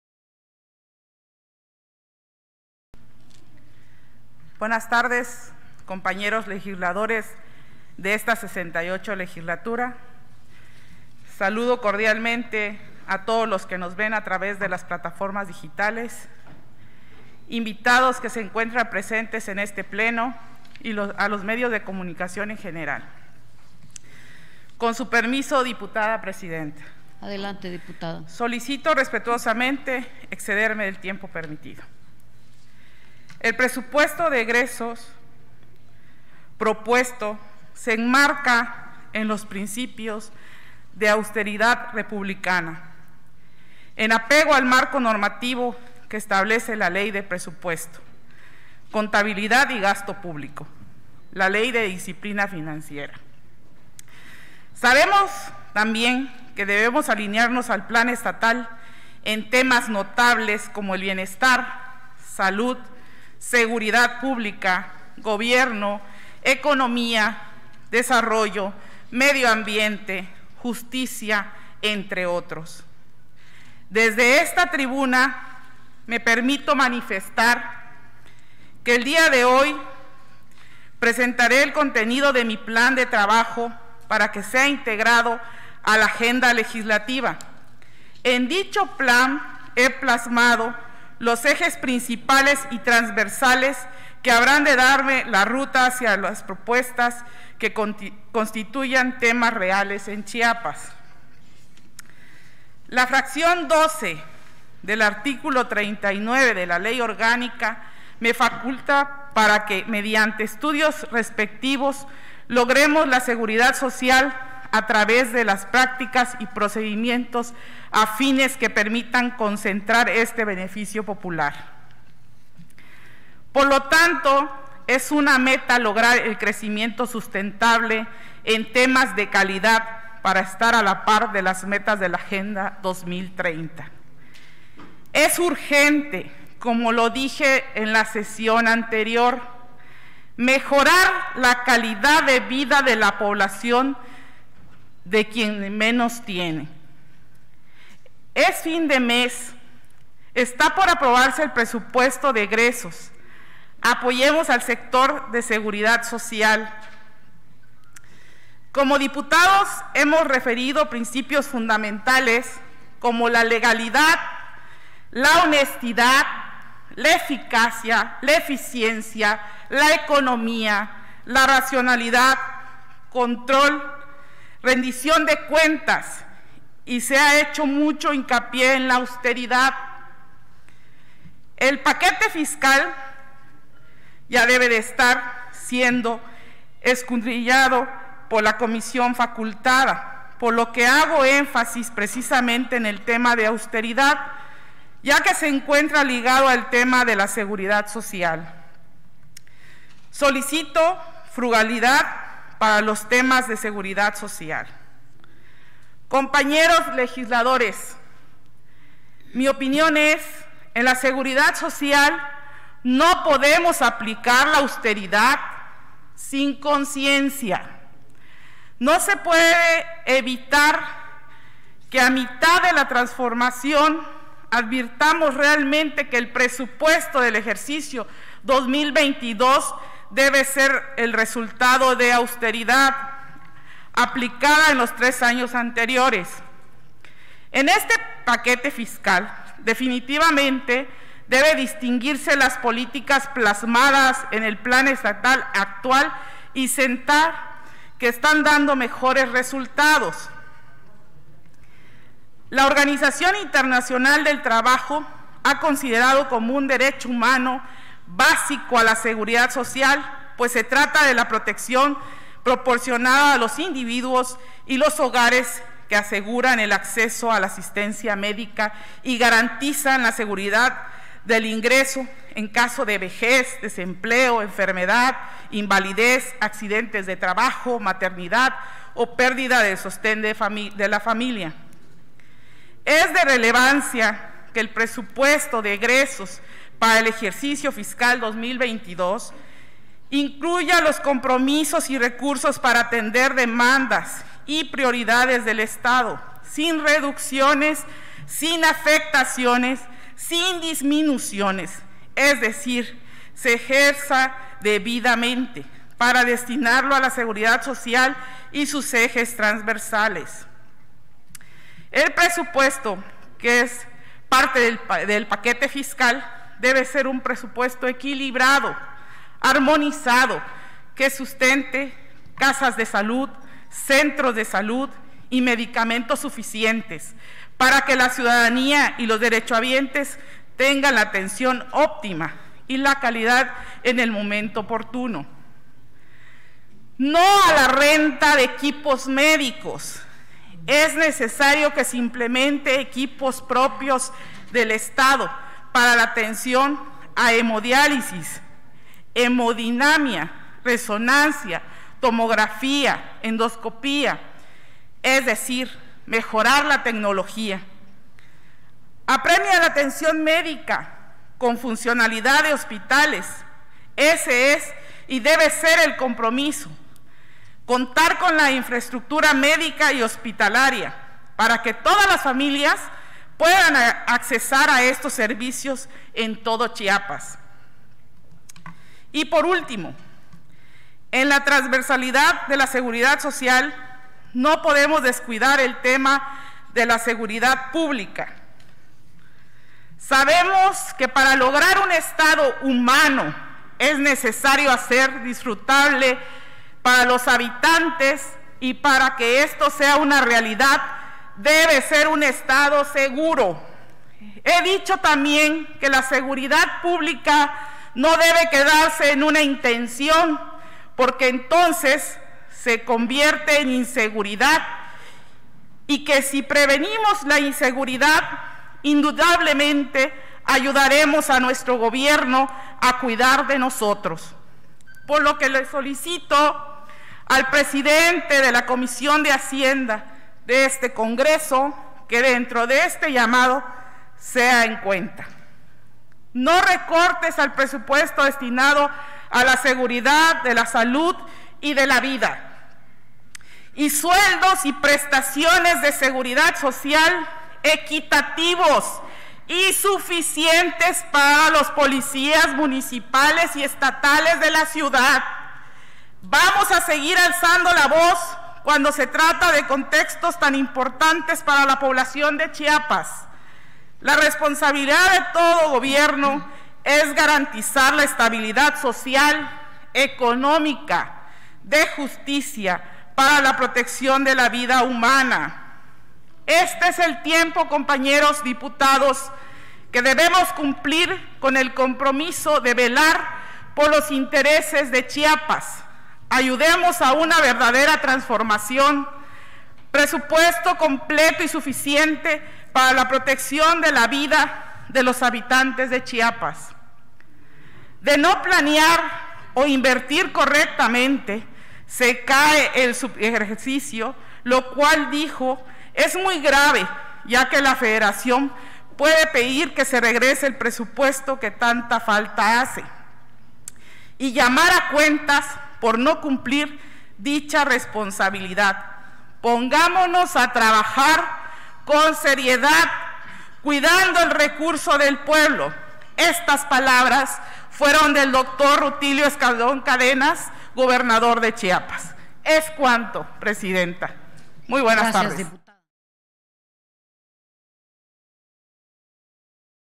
Intervención de la Dip. Sandra Cecilia Herrera Domínguez. 28 de Diciembre de 2021
Sesión de Ordinaria del día 28 de Diciembre de 2021, correspondiente al Primer Periodo Ordinario de Sesiones del Primer Año de Ejercicio Constitucional celebrada por la LXVIII Legislatura del H. Congreso del Estado Libre y Soberano de Chiapas.

Lugar: Pleno "Sergio Armando Valls Hernández"
Fecha: 28 de Diciembre de 2021
Elaborado: Unidad de Informática